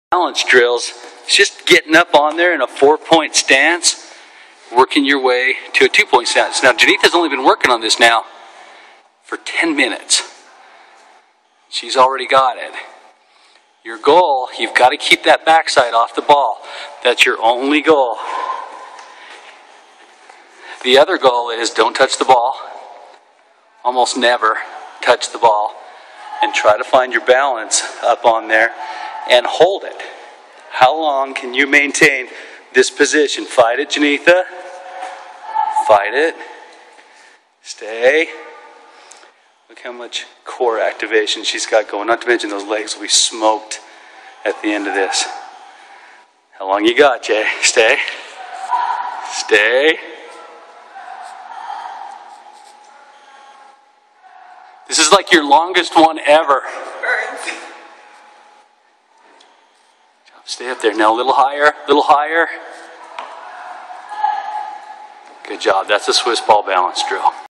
Swiss Ball Balance Drill (knees on)
Progress from the floor to a 4 point stance, then onto the knees.